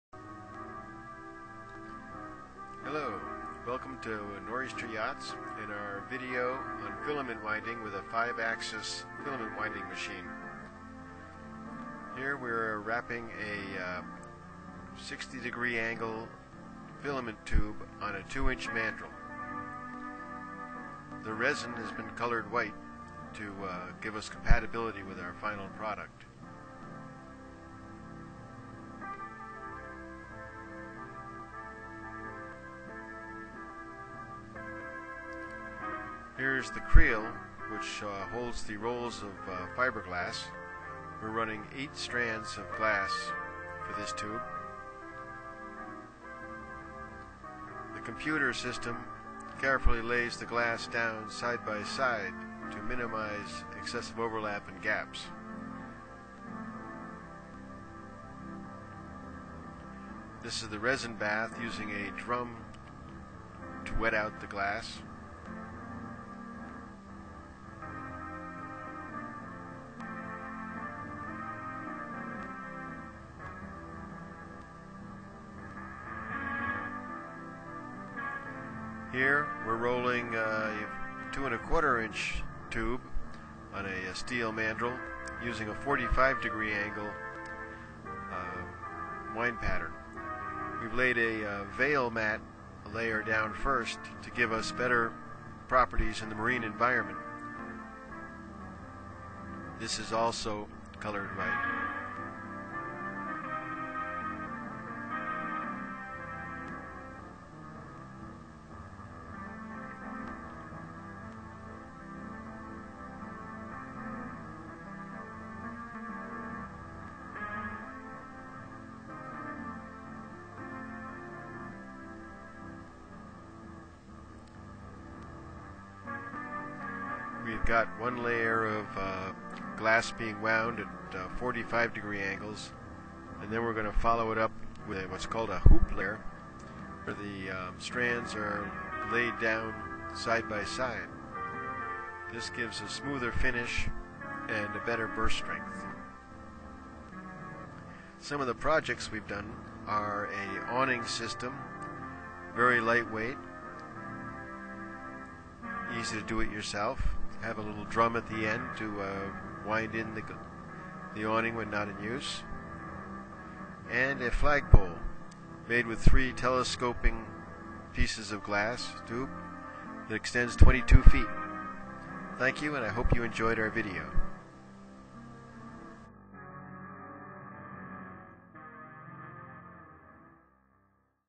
Nor'Easter X-Navy 5 axis Filament winder
Filament winding using a 5 axis Entec Composites filament winding machine demonstrating helical and hoop winding. Materials used are FIberglass and an iso polyester resin. VIdeo taken at Nor'Easter Yachts, in Milford, Connecticut.